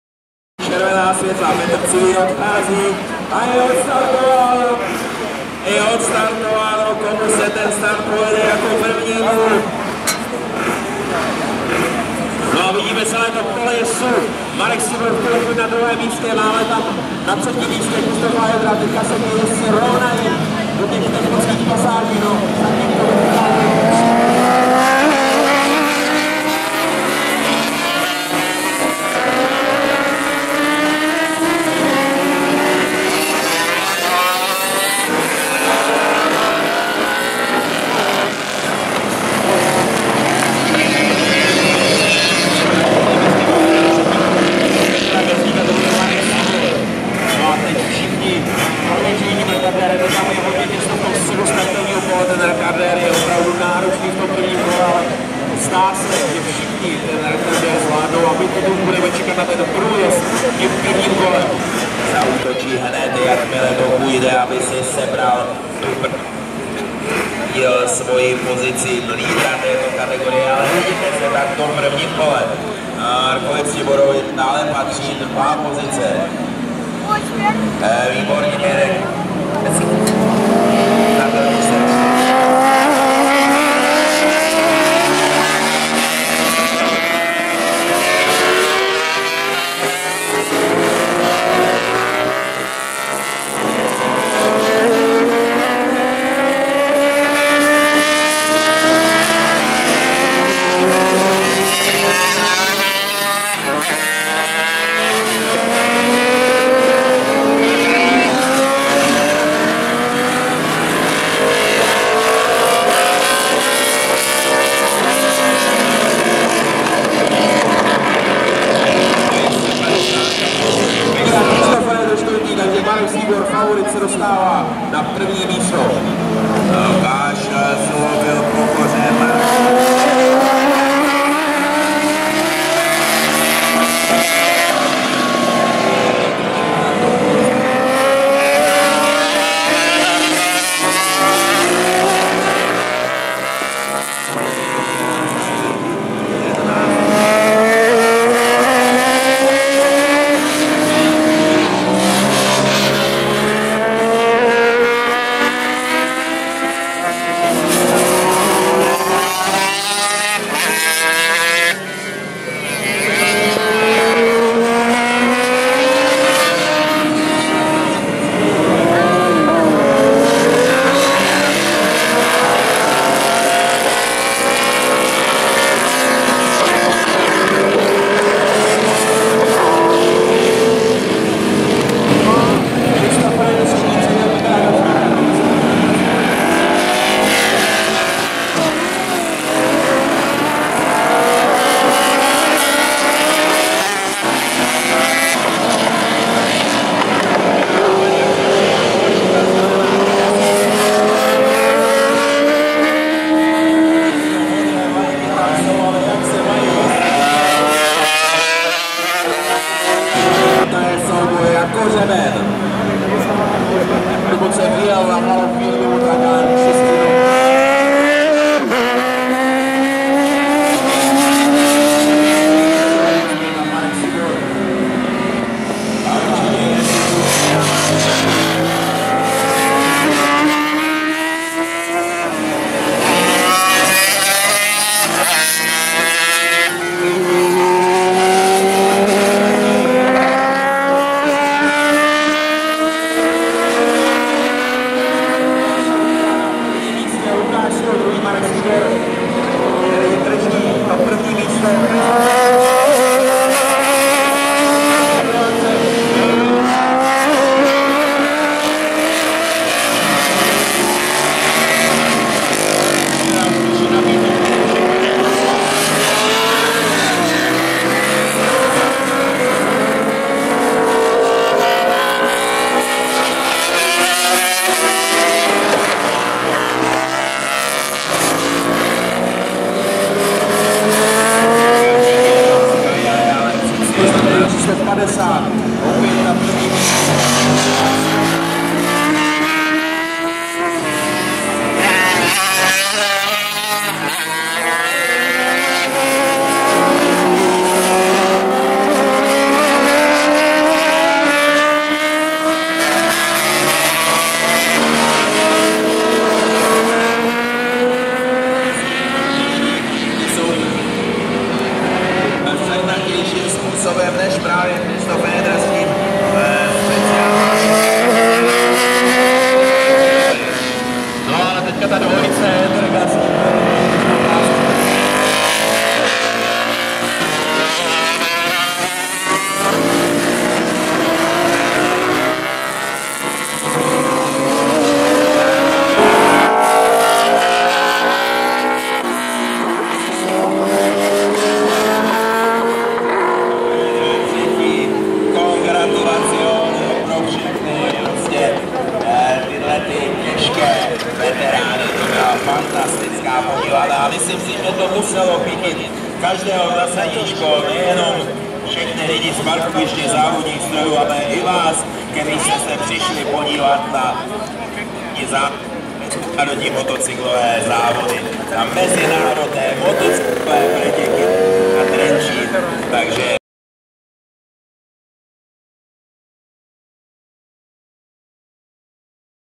Letisko Trenčín 2023 - Classic Superbike race 1
Medzinárodné motocyklové preteky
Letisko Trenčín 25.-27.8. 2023

1.závod Klasik Superbike

MČR Road Racing (CAMS + AČR)
Moto Club Baláž
SMF, CAMS, AČR